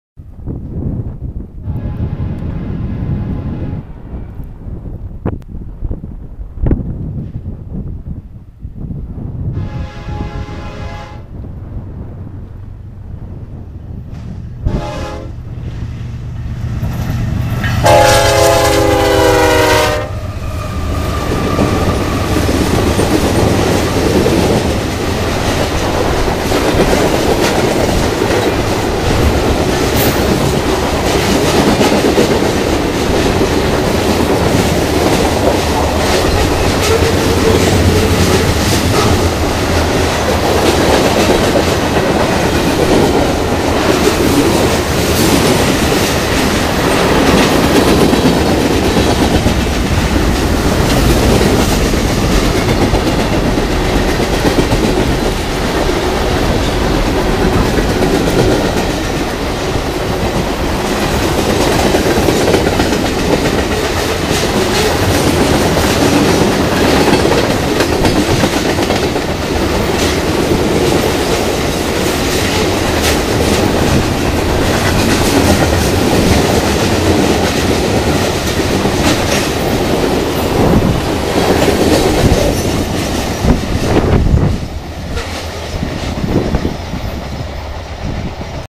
I Think Train of the Month for This One
I think this is the one!  Rockchickvw's FIRST train in her life on the Transcon is this freaking sweet lashup of a KCS ES44AC a CSX AC4400CW and a CSX SD50!  Wowzers.